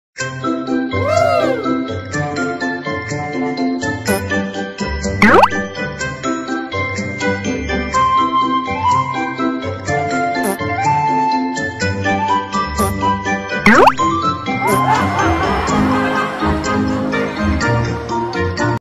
Oc mới đã đến. Mimi cùng với RosyClosy||Gacha club||by: Mimi||Like+Share+Subs+Cmt. TU
Đây là tui khi mà cùng với RosyClozy kiểu:
Mimi: Chào Rosy, do là máy tui lag nên nói đc ít.
Rosy: Trời, tự nhiên cái thoát, thoai thoát ra còn làm thêm ep.12 TMF cho các bạn nữa:)
                           The end